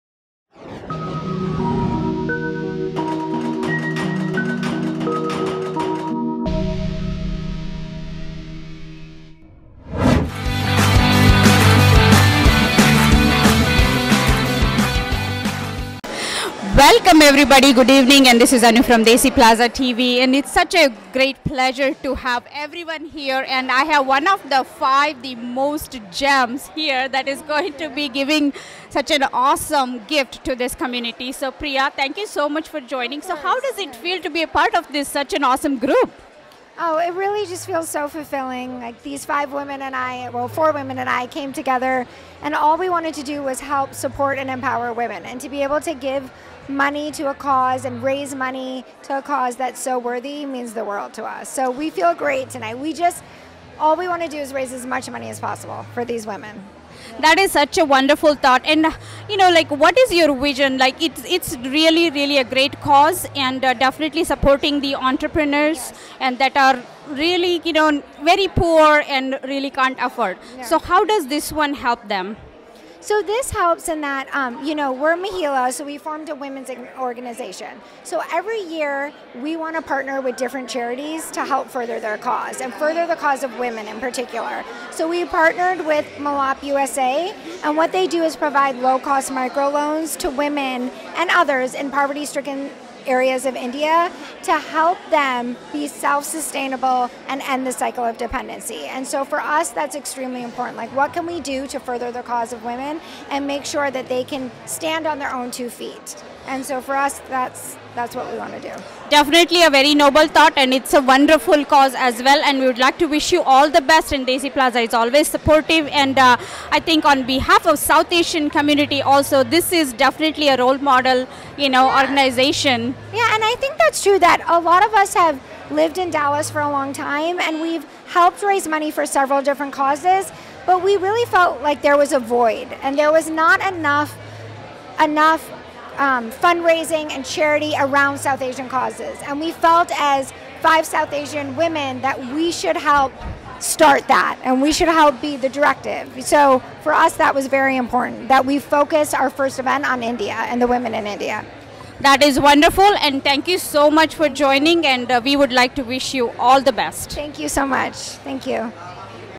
Priya Rathod Speaking to Desiplaza TV about the fundraising done by Mahila.
(Dallas, Texas – April 11, 2016) With the beautifully-lit Dallas skyline as the backdrop, more than 300 of Dallas’ crème-de-la-crème, from businessmen to financiers to fashionistas, packed the d.e.c. on Dragon in downtown Thursday evening and raised more than $85,000 for Mahila, an organization comprised of powerful female community leaders: attorney Bina Palnitkar Patel, architect Purva Jain, media guru Priya Bhola Rathod, cultural advocate Preeta Monga and social entrepreneur Ami Doshi.
PHOTO CREDIT FOR ALL IMAGES: SHANA ANDERSON
This was our very first fundraiser, and I’m quite pleased at the turnout,” said founding member Monga. “Dallas is a very giving community, and we are thrilled that people in our city resonate with this international cause. I have to thank all of our incredible monetary & in-kind sponsors, the community and, of course, my fellow ‘Mahilas,' without whom none of this would be possible.”

All the proceeds raised will directly benefit struggling, low-income village women in India who have a dream of starting their own business throughMilaap USA, the beneficiary of the fundraiser.  The non-profit strives to provide low-cost microloans to the working poor to empower them to live dignified, self-sufficient and healthy lives. Through microlending, women receive small business loans which are repaid over a period of time. Milaap USA advocates financial inclusion for individuals and families in the following sectors: enterprise development, clean energy, water and sanitation, education and vocational training and health care. Watch the Milaap video HERE!

“We are so humbled and honored that Mahila picked us as their charity of choice for their inaugural fundraiser,” said Paulomi "Polo" Patel, Executive Director of Milaap USA. “Our organization is driven to provide a ‘hand-up,’ versus a ‘hand-out,’  to the women we work with in India. We know if that if you mobilize ONE woman, you help mobilize her family, and, ultimately, her community; these rural women are leading the next generation out of poverty. It’s an investment worth making as global citizens.”

To support the cause, ‘Real Housewives of Dallas’ star LeeAnne Locken andSatnam Singh, the first player from India to be drafted into the NBA by the Dallas Mavericks, also attended the event and walked the red carpet.

North Texas Preferred Health Partners
Ericsson
Pratiksha Jewelry
Competitive Cameras
CMC Commercial Reality Group
Monitoring Concepts
Source : Jingo Media